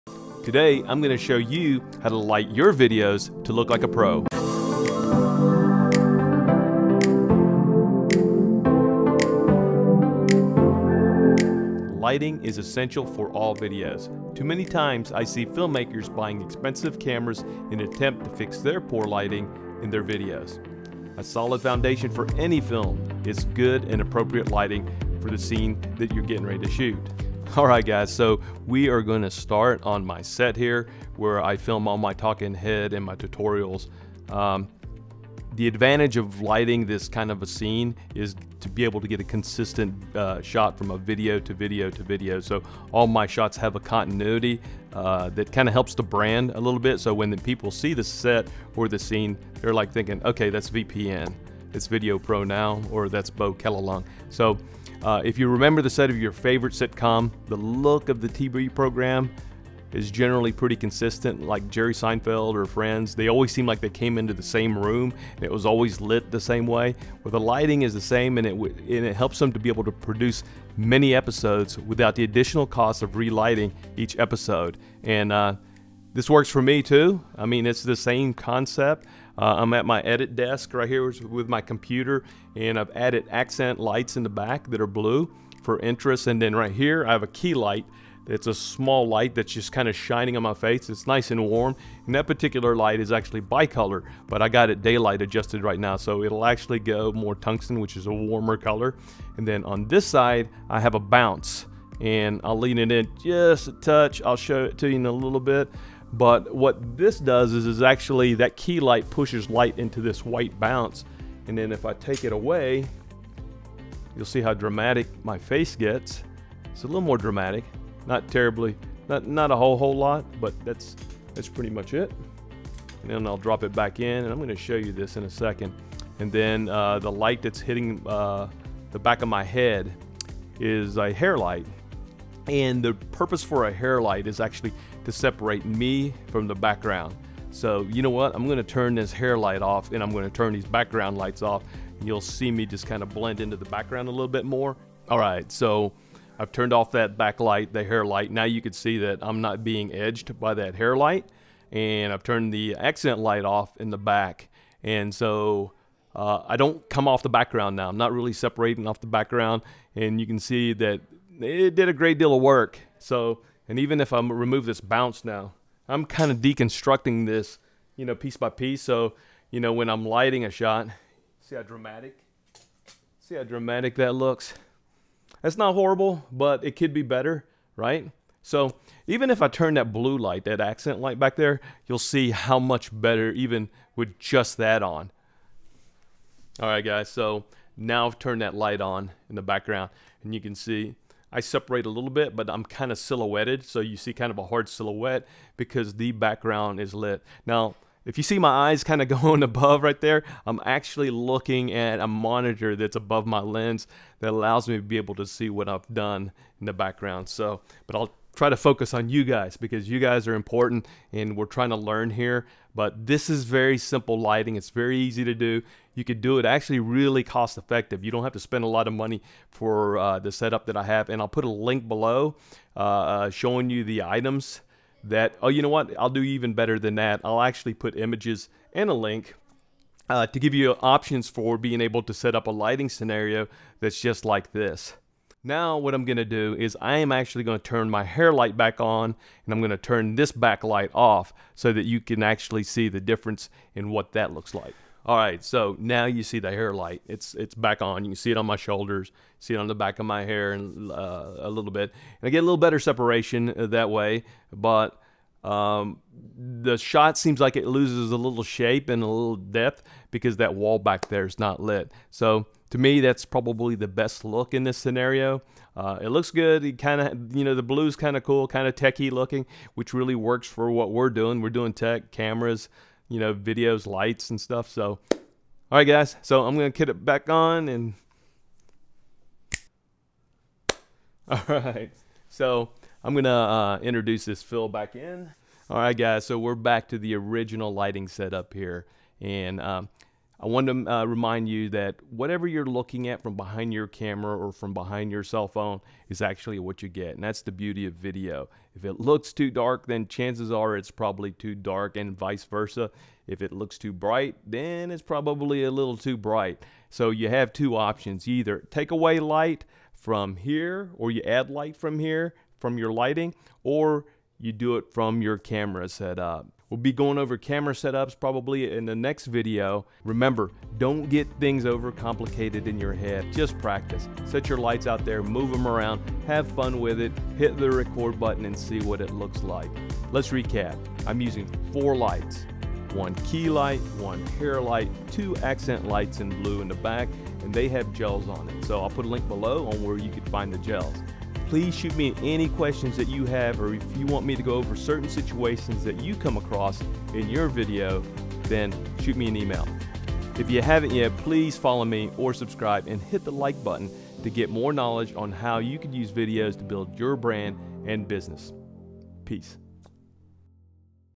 How to light for your Youtube Channel.
Watch as I show you how to light and film a video interview for beginners.

Link to the light kit